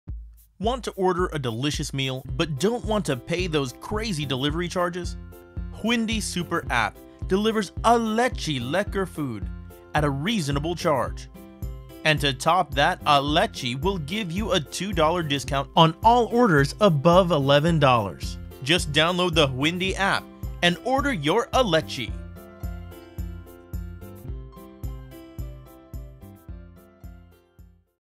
Alechi Discount
Want to order a delicious meal but don’t want PAY those crazy delivery charges?

Hwindi super app delivers ALECHI lekker food at a very reasonable charge.
And to TOP that ALECHI will give you a $2.00 discount on all orders above $11.00!